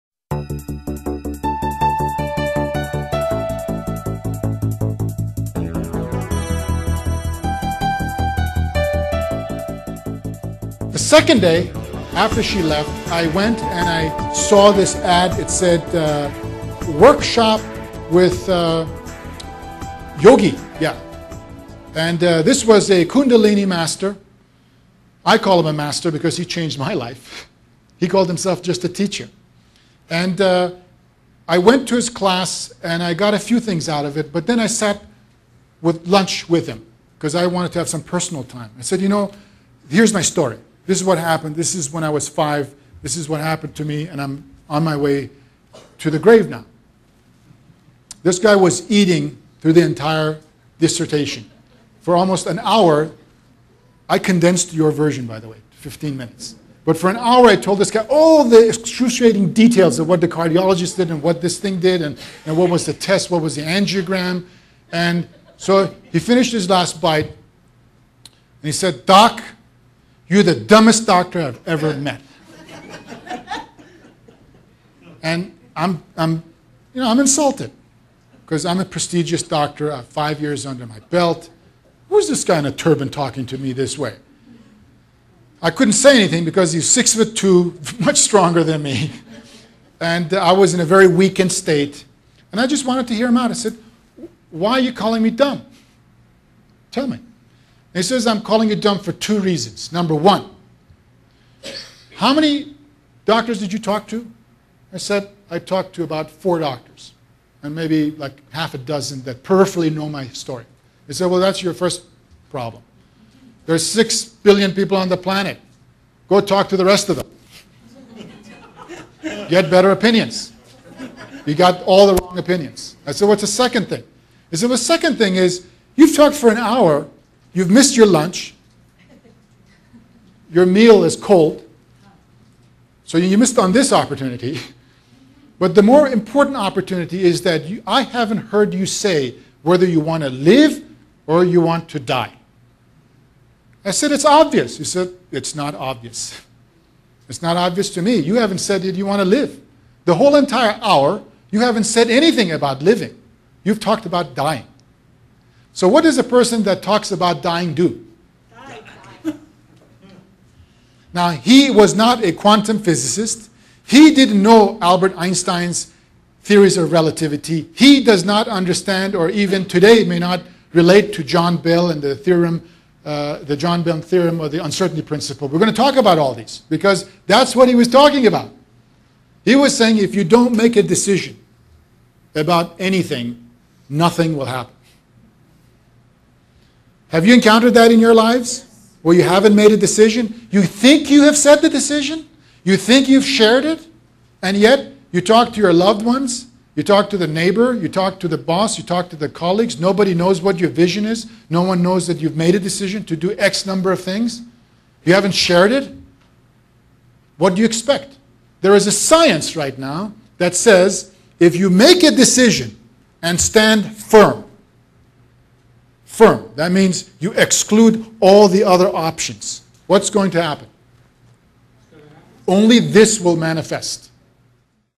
Dr Noah tells his story Part Two
Dr. Noah McKay, M.D. personally encountered the limitations of conventional medicine when he was hospitalized in acute heart failure at the age of thirty-three. He spent the next two decades exploring the converging disciplines of quantum physics, spirituality and alternative medicine.

See him tell his story, part two.